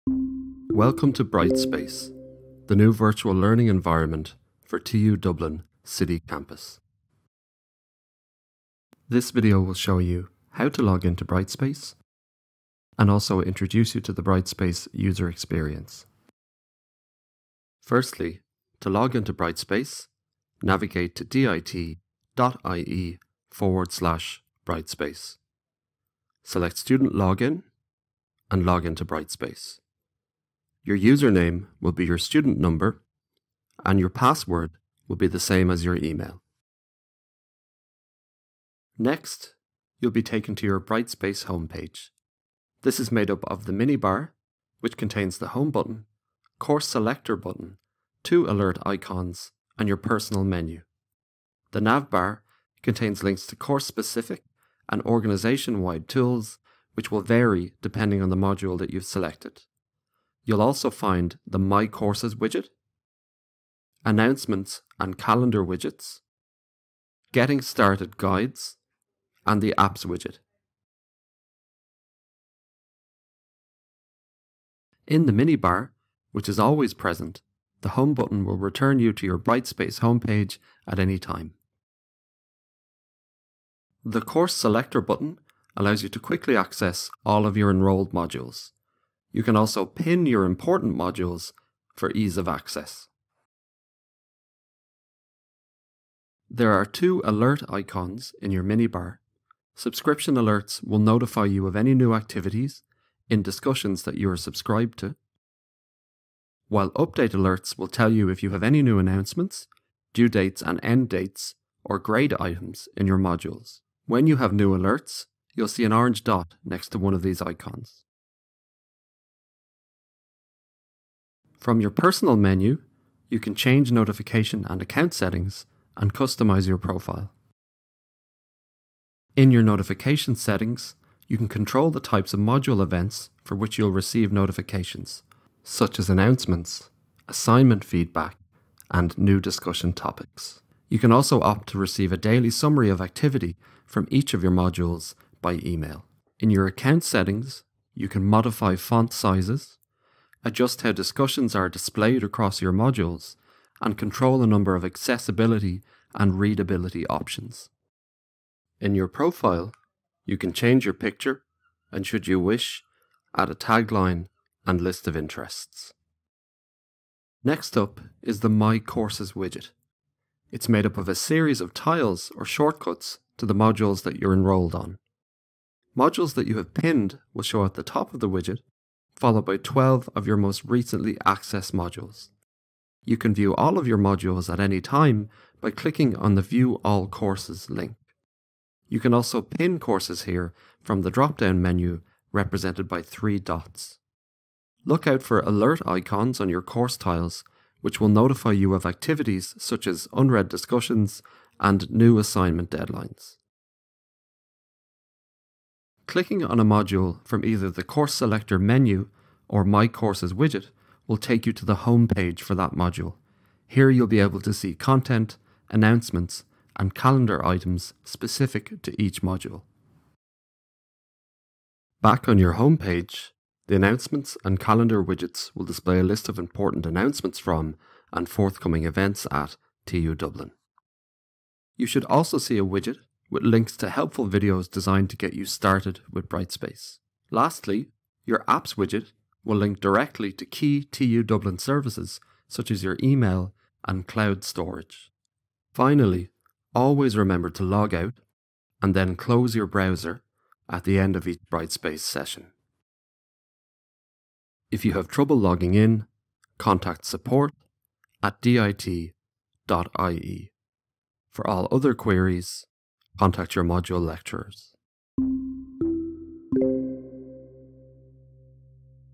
Bite-sized Brightspace for Students No. 1 - 'Getting Started'
Part one of the 'bite-sized brightspace' series for students at TU Dublin City Campus.

In this video you will learn how to:

1) How to log in to brightspace
2) How to navigate the brightspace user interface